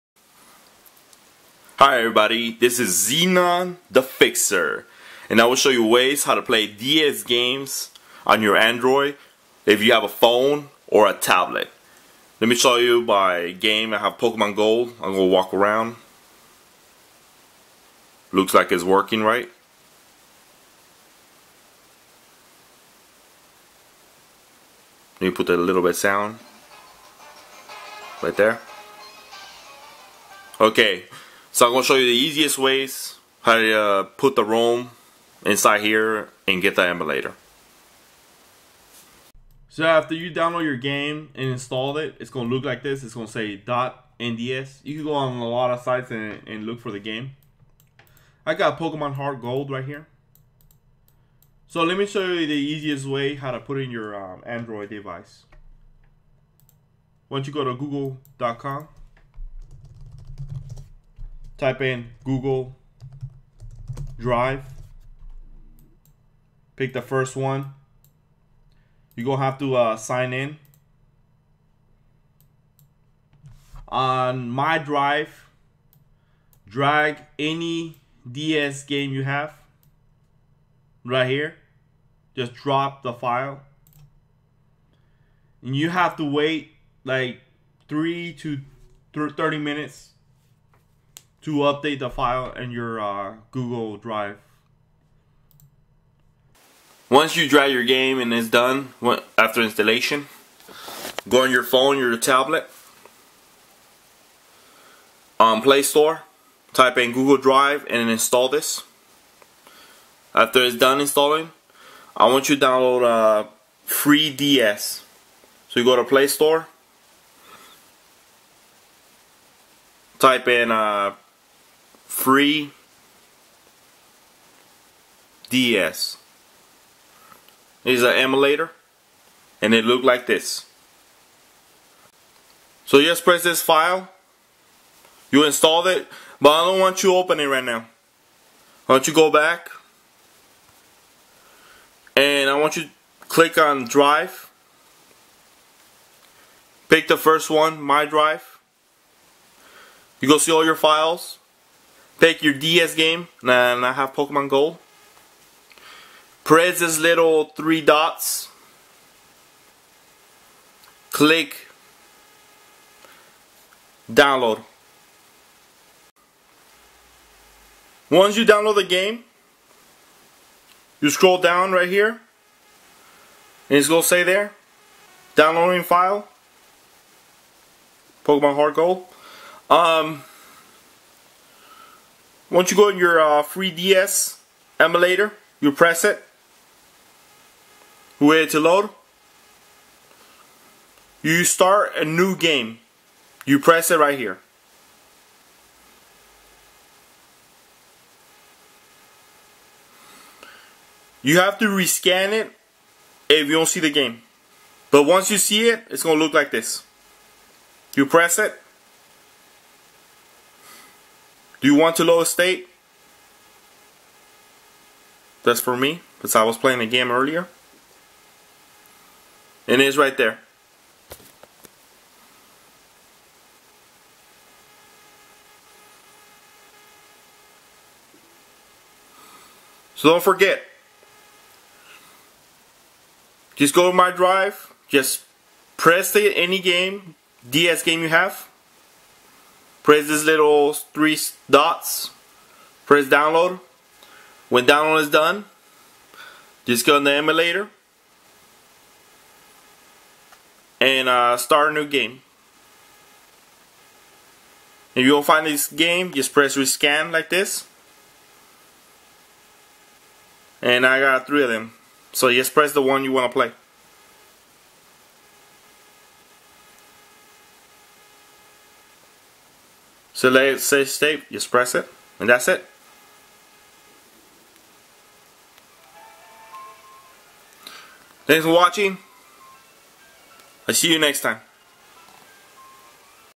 HOW TO PLAY DS ROMS ON YOUR ANDROID DEVICE WITH CLOUD STORAGE
Want to emulated your DS games on your android phone or tablet i will show you a way how to fix it.